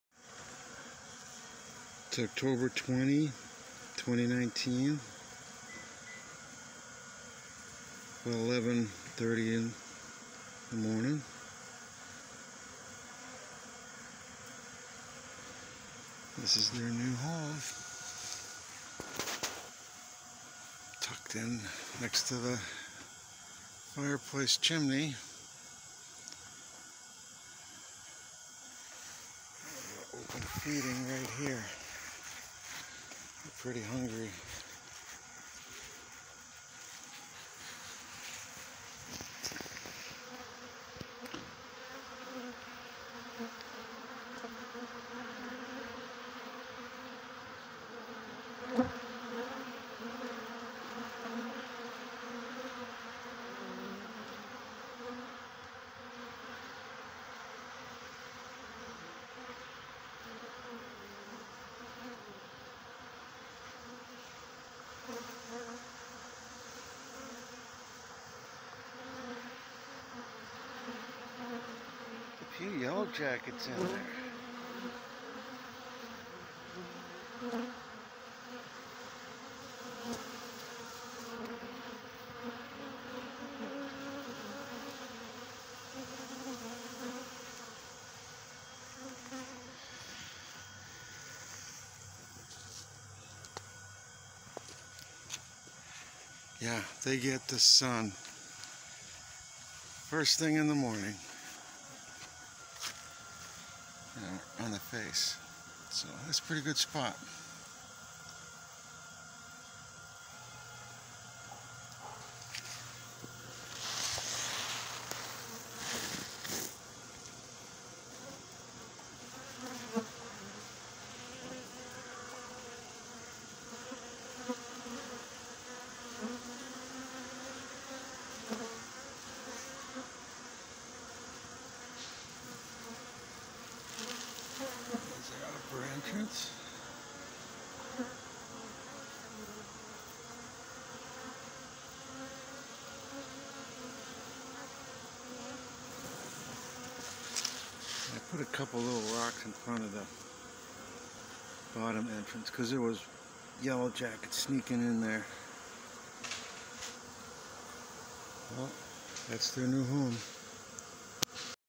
My bees new home for now
My bees live in their hive made by stacking up a pair of five frame deeps.  They have survived a road trip of over 3000 miles, from Whidbey Island to Massachusetts, stuck in that hive,loaded into a small cargo trailer.